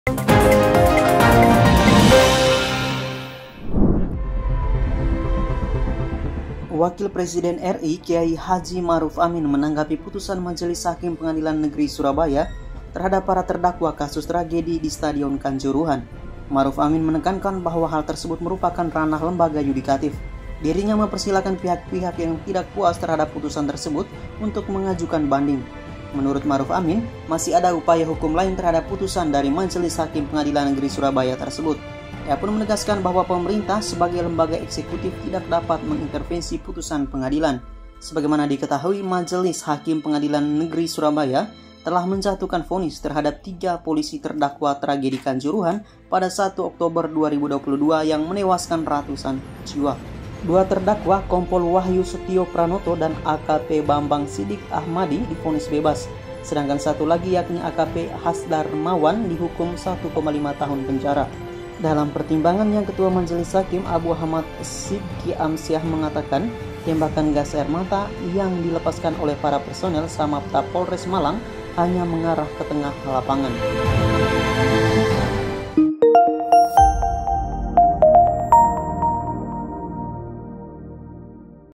Respons Ma'ruf Amin Sikapi Vonis Bebas Terdakwa Kasus Kanjuruhan
Wakil Presiden KH Maruf Amin menanggapi Putusan Majelis Hakim Pengadilan Negeri Surabaya terhadap para terdakwa kasus tragedi Stadion Kanjuruhan. Maruf Amin menekankan bahwa hal tersebut merupakan ranah lembaga yudikatif.
Dirinya mempersilakan pihak-pihak yang tidak puas terhadap putusan tersebut untuk mengajukan banding. Menurut Maruf Amin, masih ada upaya hukum lain terhadap putusan dari Majelis Hakim Pengadilan Negeri Surabaya tersebut. Ia pun menegaskan bahwa pemerintah sebagai lembaga eksekutif tidak dapat mengintervensi putusan pengadilan.